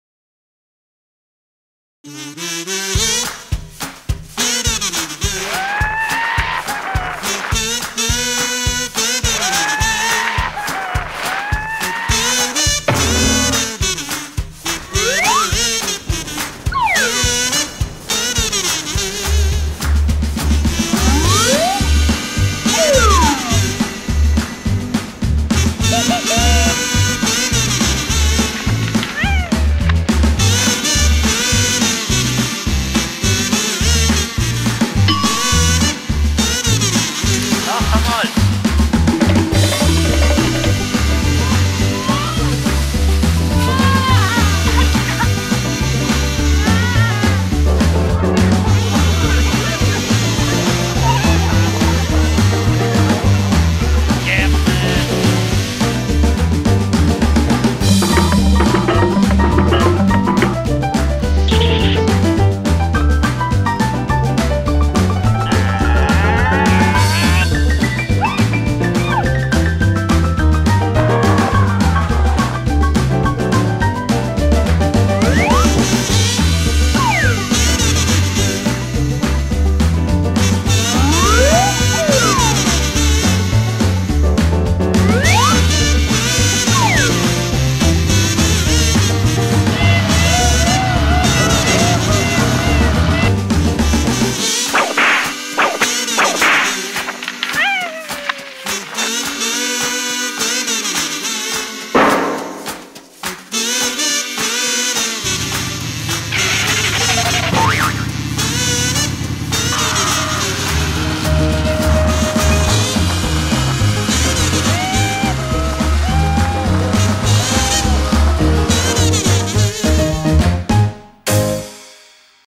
GoPro HERO 4 Little Planet
Snowboard and Freeski Edit, made with 6 GoPro Hero 4 stitched with Autopano Video Pro, and Autopano Giga